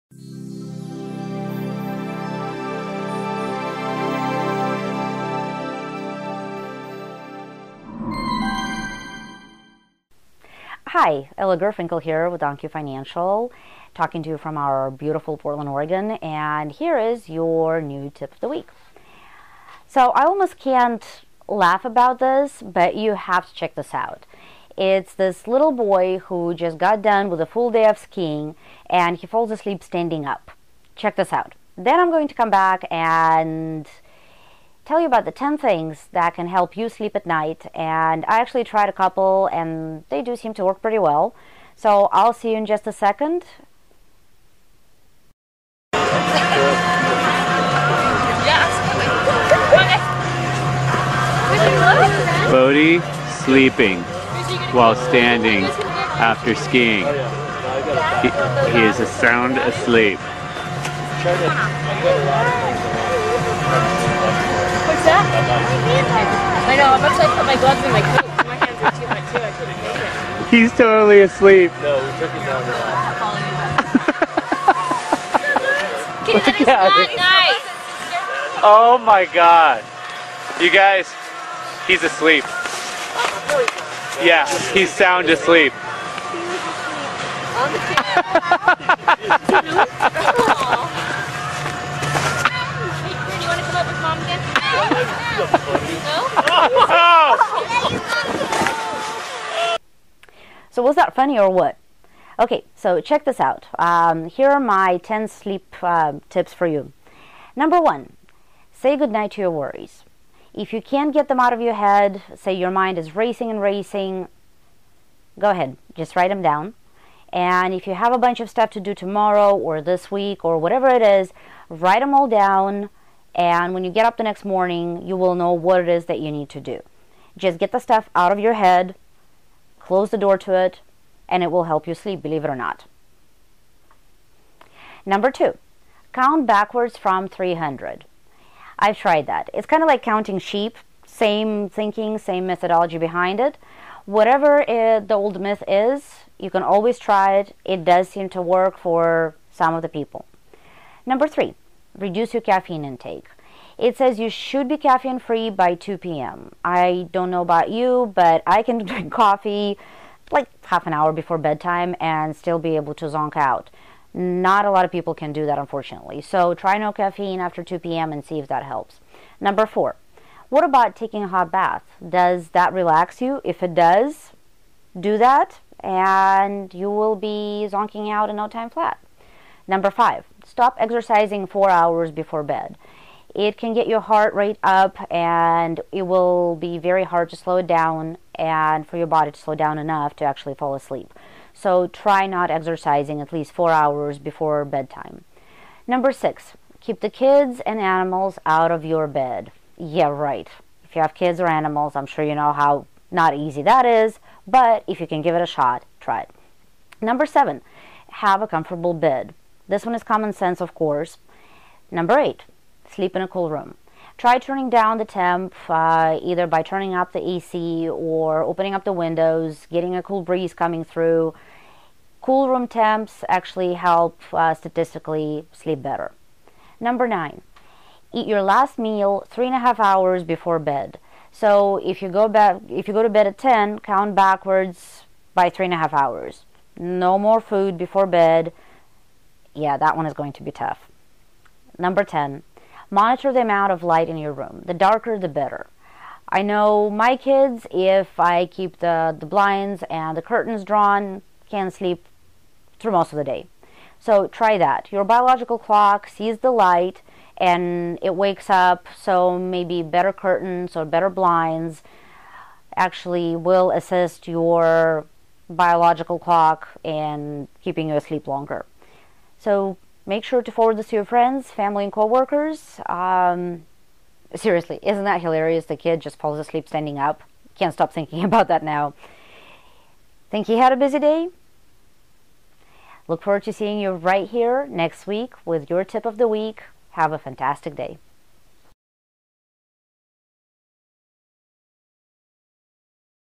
Tip of the week: Good Night's Sleep
This week Ella Gurfinkel shares some really great tips for getting a good night's sleep. Watch the video to find out more!